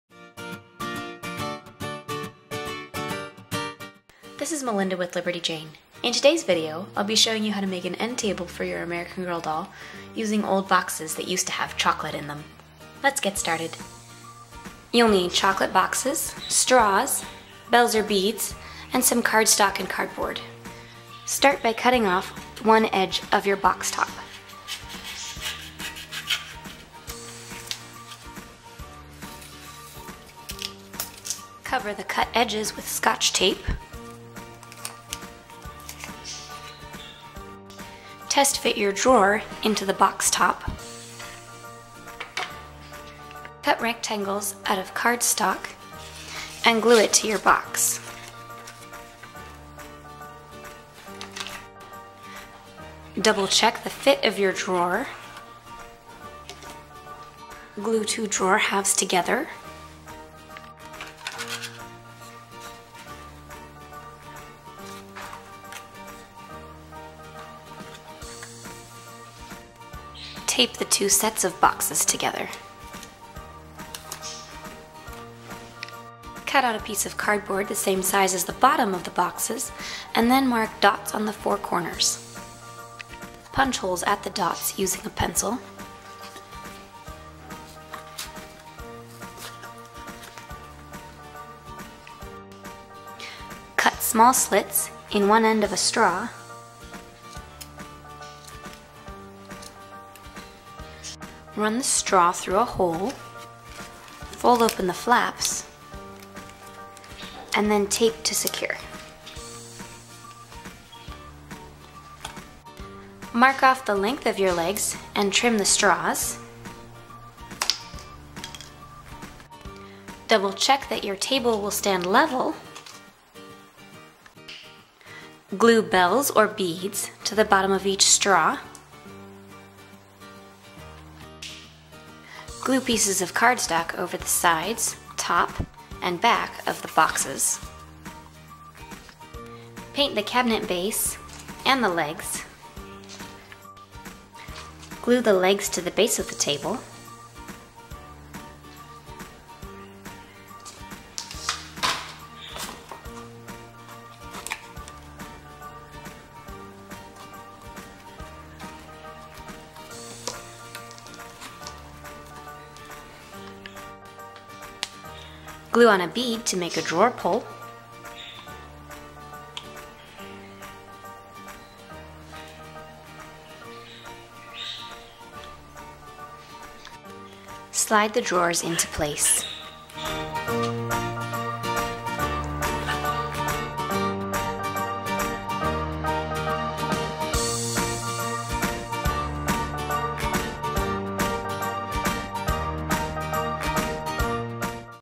How To Make an Doll Size End Table for American Girl Dolls
Follow along as Melinda from Liberty Jane shows you how to take tiny chocolate boxes and turn them into a cute set of drawers for your 18 inch doll!